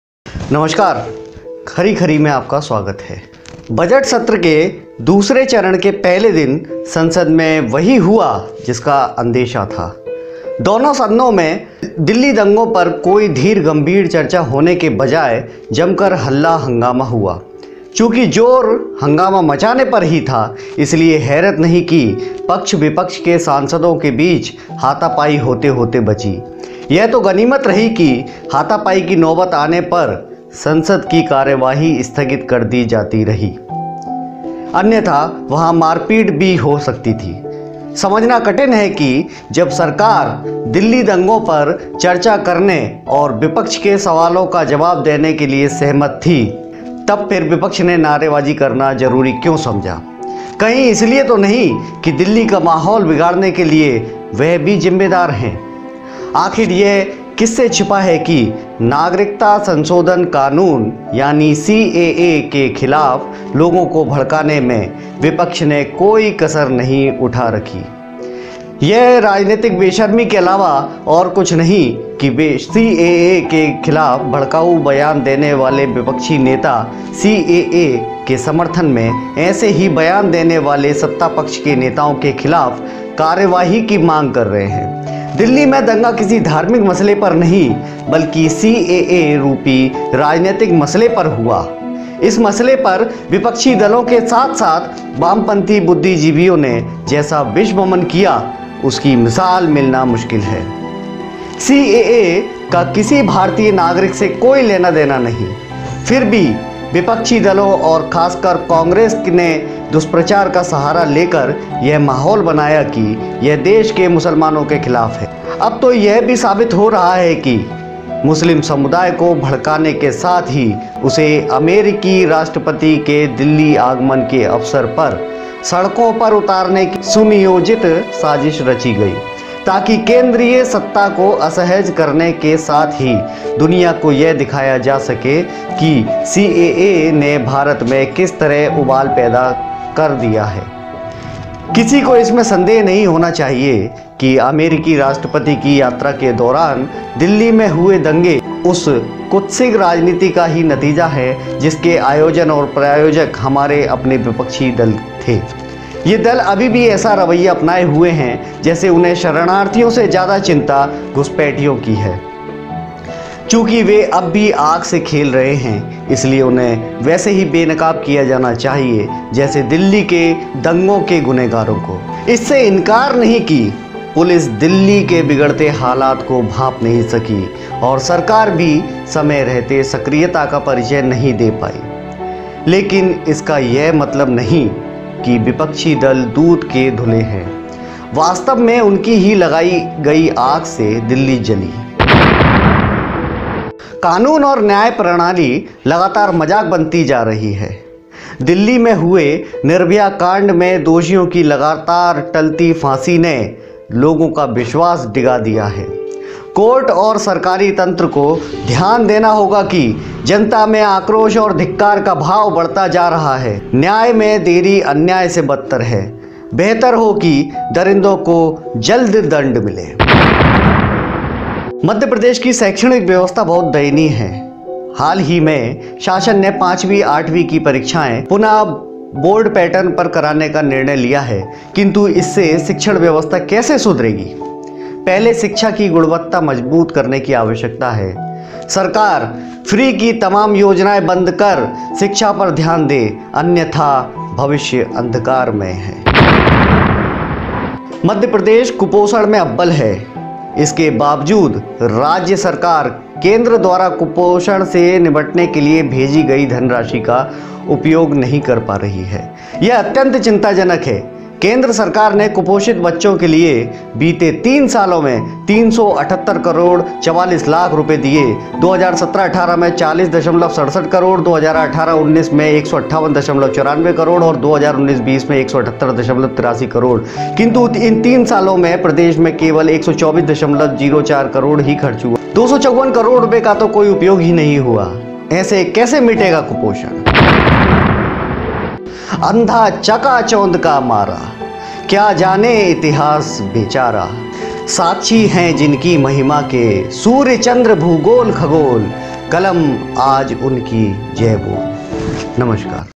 आज की खरी खरी
*दिल्ली का माहौल बिगाड़ने के लिए जिम्मेदार विपक्ष
*दरिंदों पर इतनी दया भी ठीक नहीं
*अन्धकार में डूबी है सरकारी शिक्षा
*म.प्रदेश पर कुपोषण का दाग कब तक